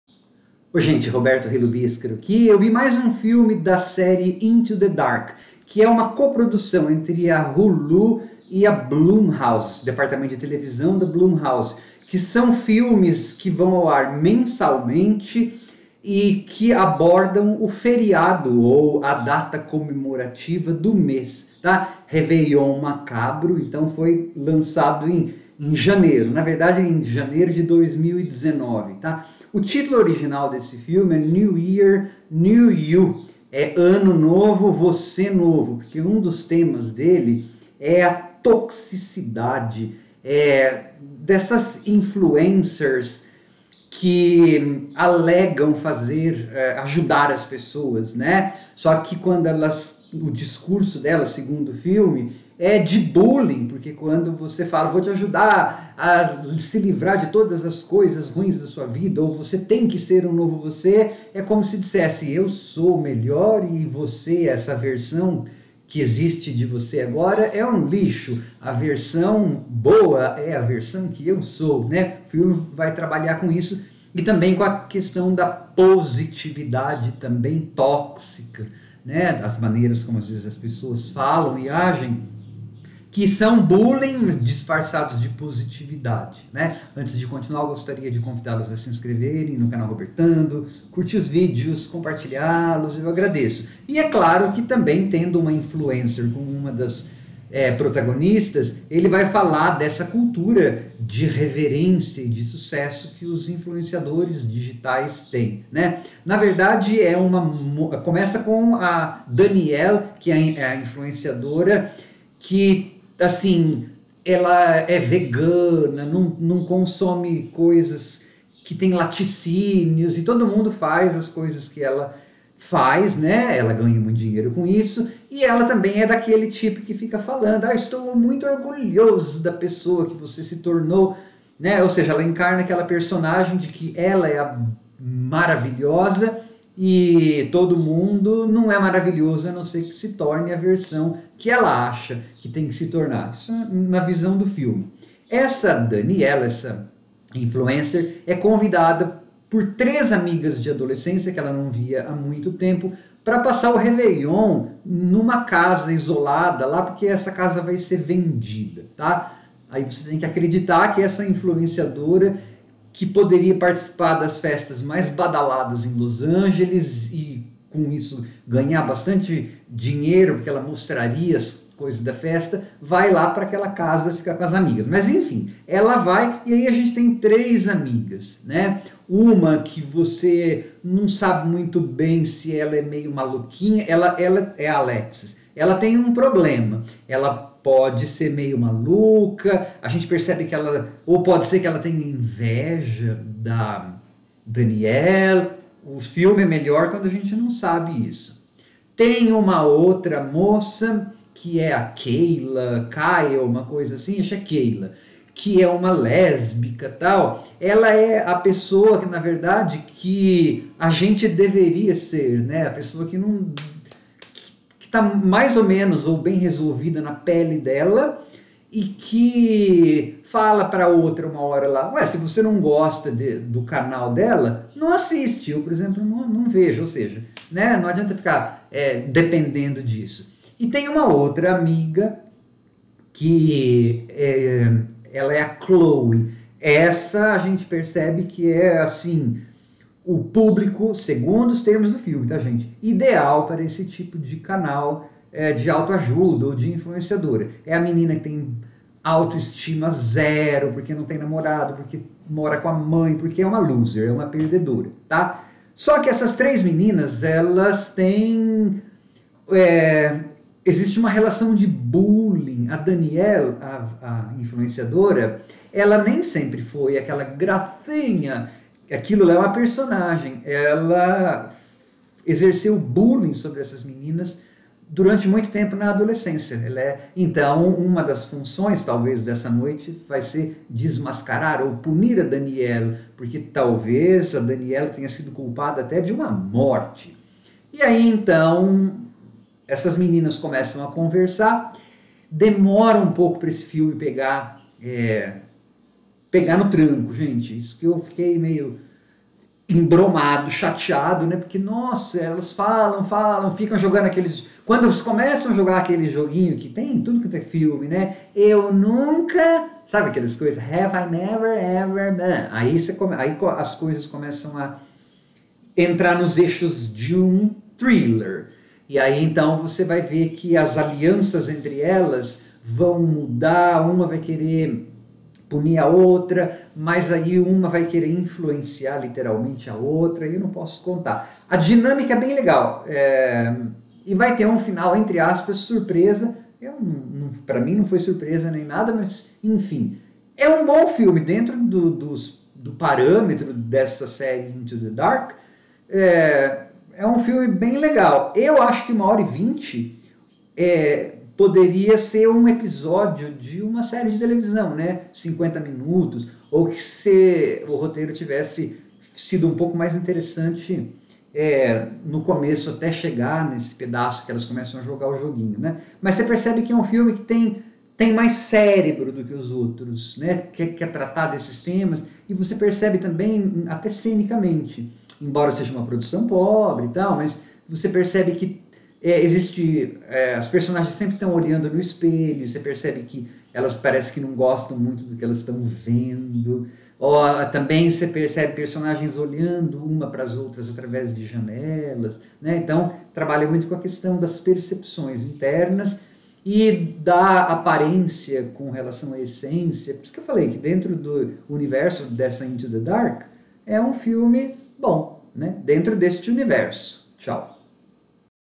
Crítica do filme Into the Dark: Réveillon Macabro (HBO Max)
Quatro amigas se reúnem para a virada do ano. Uma delas é uma famosa personalidade da internet, o que deixa as outras com uma certa inveja.
Zona Morta grupo do Facebook dedicado a amantes do horror, suspense e afins.